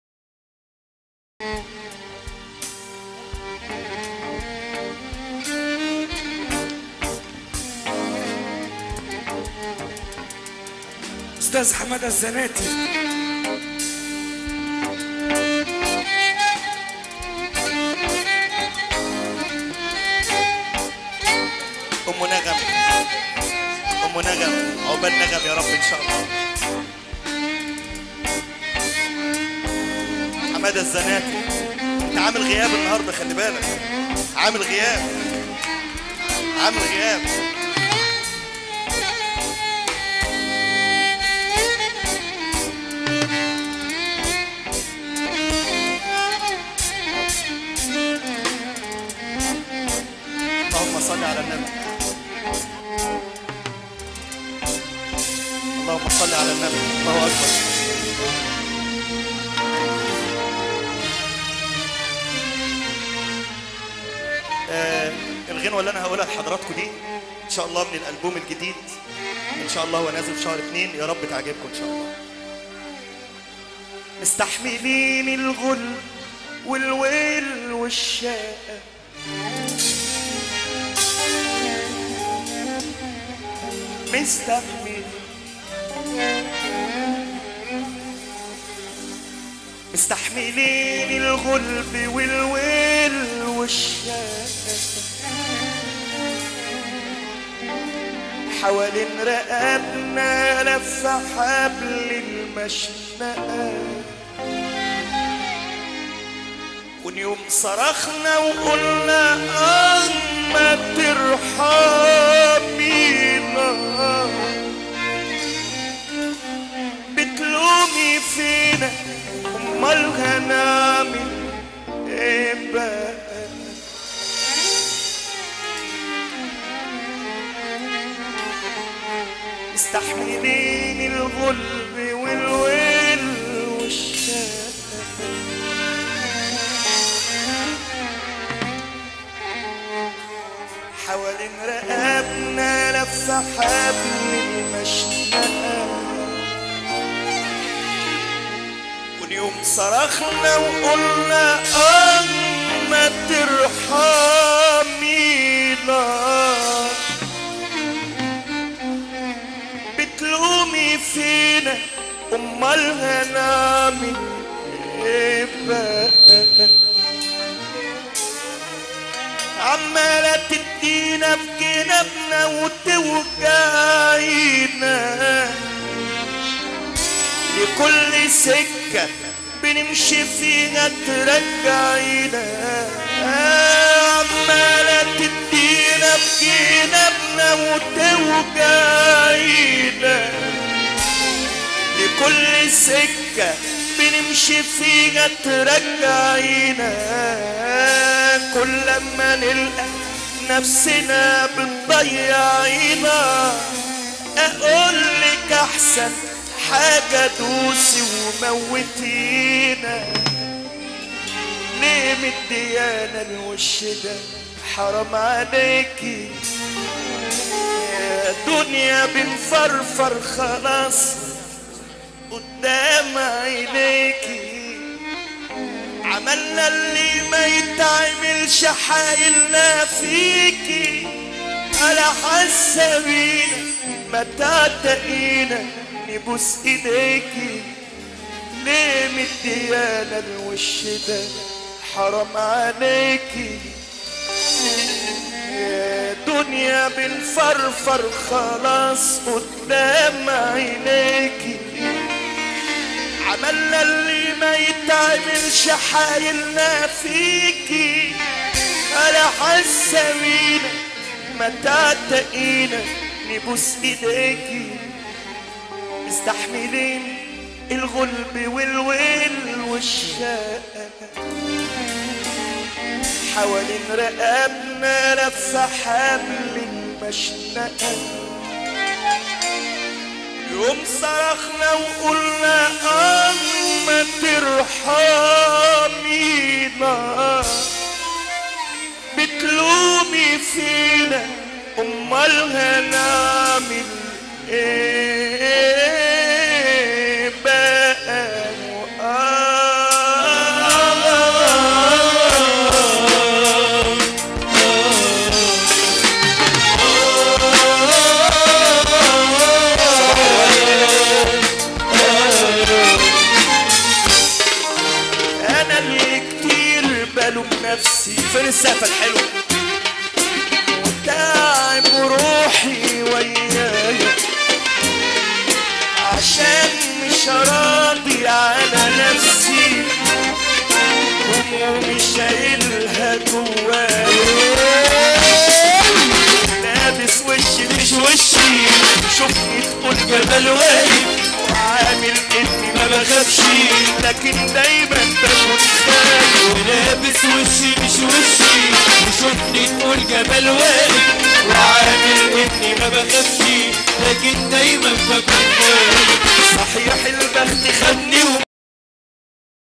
النجم سمسم شهاب من فرحة محمد عبد السلام دمار شامل نجوم مصر الافراح المصرية 2016
عبد السلام مولع فى الفرح بتاعة ومجنن الناس
سمسم شهاب وعلا محمد واسماعيل الليثي واحمد الاسمر ومصطفي حجاج على أنغام الموسيقار محمد عبدالسلام مولعين الدنيا ورقص ودلع وهو ده الحظ فرح محمد ملوك في المعصرة بلقاس.
رقص بنات شعبى فى الشارع على مهرجانات,
رقص بنات على مهرجانات,
رقص بنات شعبى فى البيت,